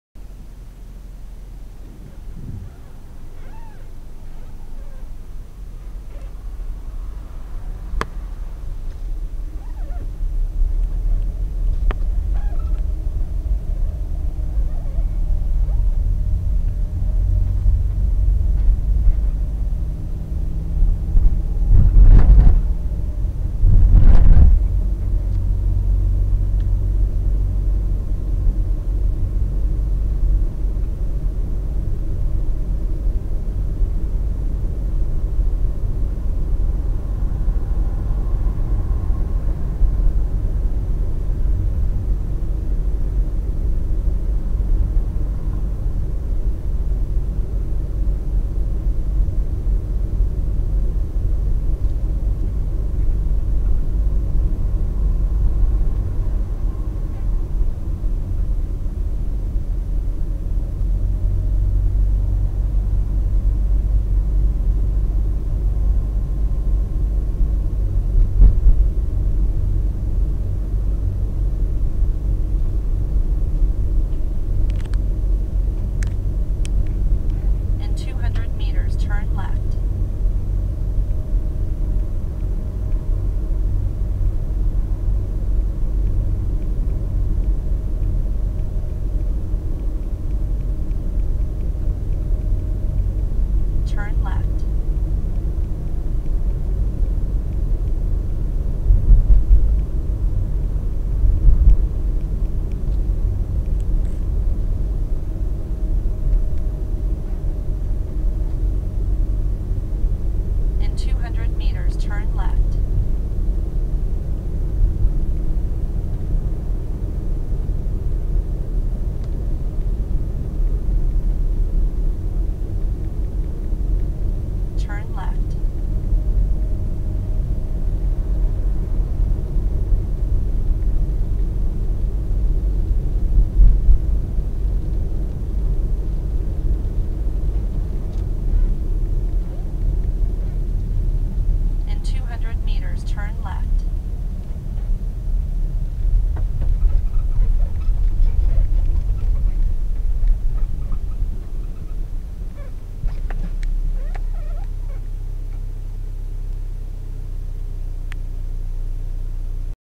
A drive through Wendell, Idaho
Wendell is located in Gooding County, Idaho at an elevation of 1,046 metres (3,432 feet). Wendell is located along Interstate 84 at about 390 kilometres (240 miles) northwest of Salt Lake City, Utah. At the time of this recording Wendell had a population of about 2,700.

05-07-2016